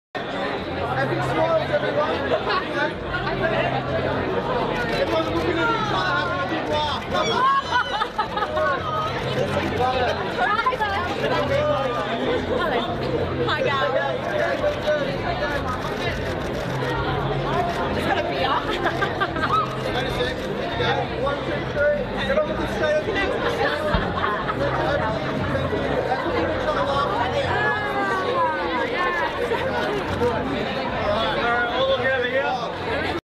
Former Bachelor contestants stun on Maxim Hot 100 red carpet
Megan Marx, Tiffany Scanlan, Bec Chin and Noni Janur party together in Sydney